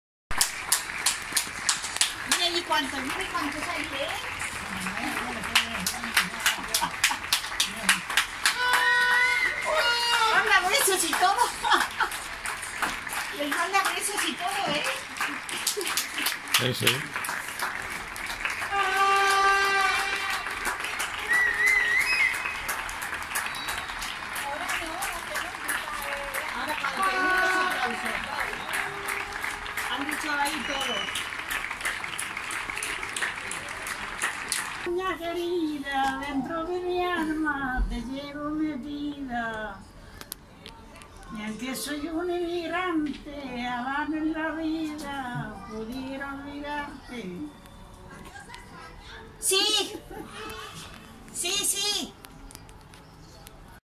Aplausos para una mujer de 100 años que supera el coronavirus
Vecinos de Barcelona han recibido con aplausos a Micaela, una mujer de 100 años, que ha regresado a su casa tras superar el coronavirus.